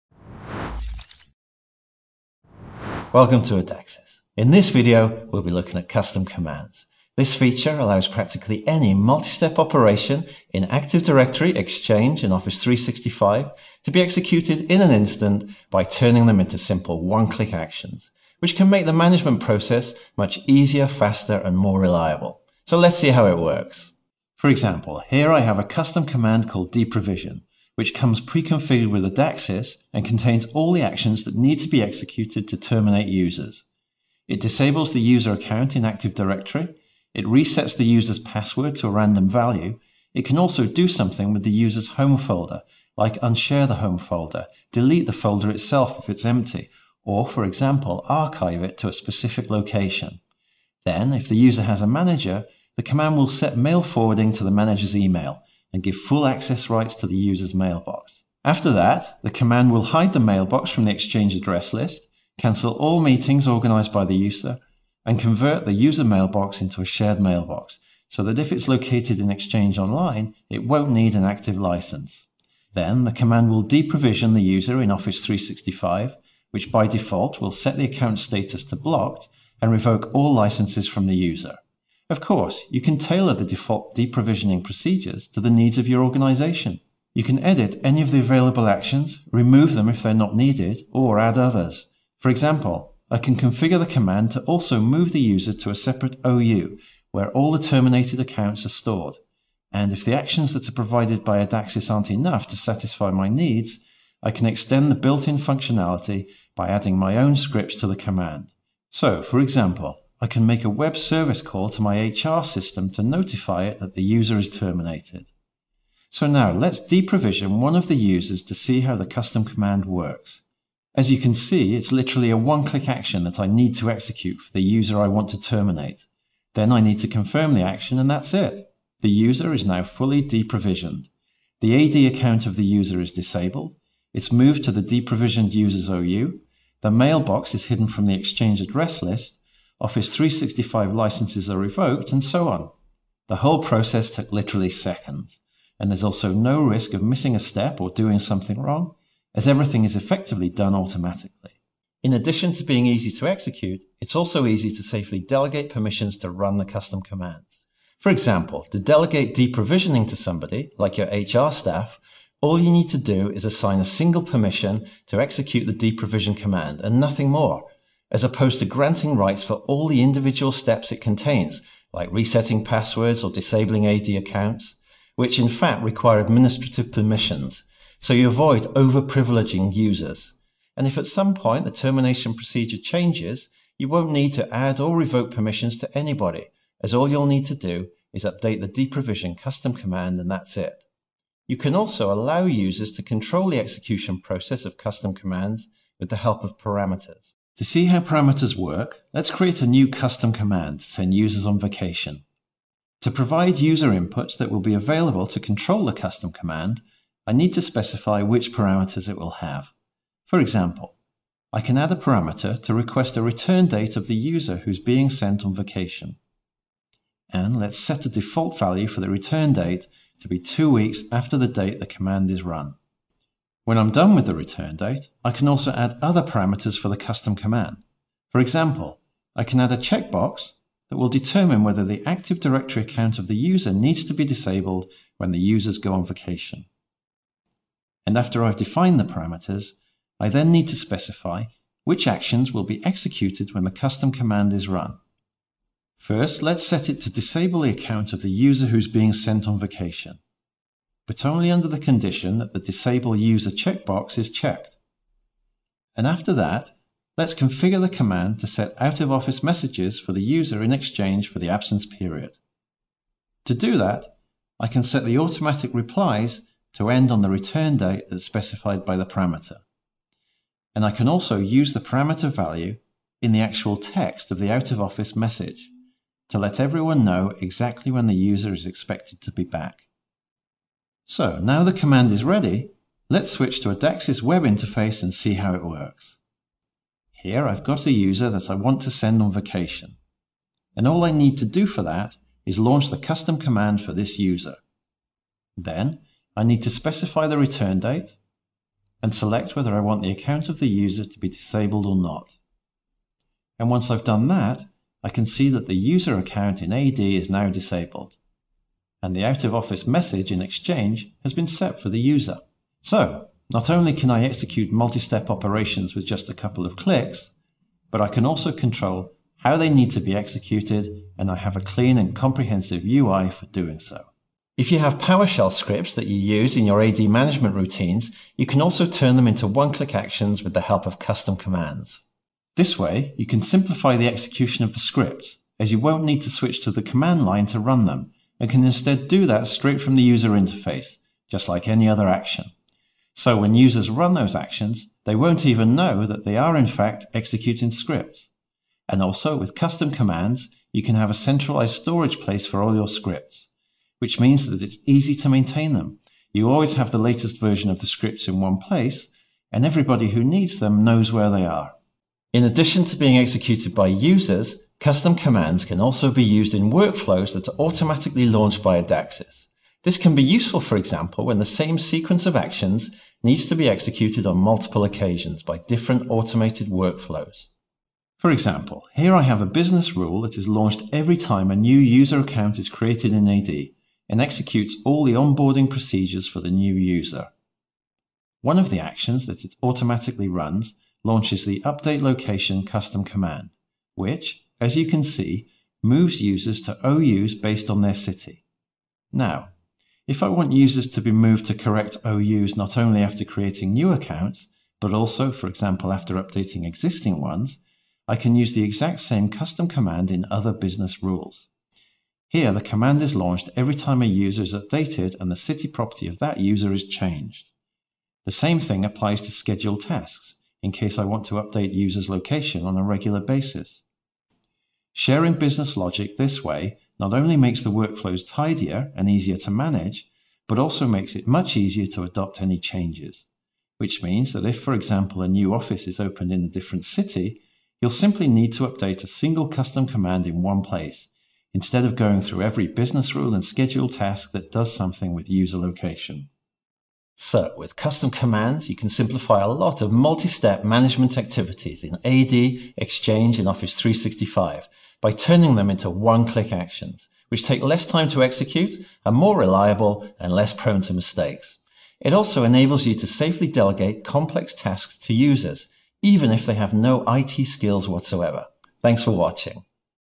Custom commands for Active Directory, Microsoft 365 and Exchange | Adaxes
Learn how to turn complex, multi-step operations in AD, M365 (O365) and Exchange into simple one-click actions.

For example, with the help of built-in Deprovision command, you can convert the offboarding process into a single action. Or, using custom commands, you can allow users to execute PowerShell scripts right from the user interface.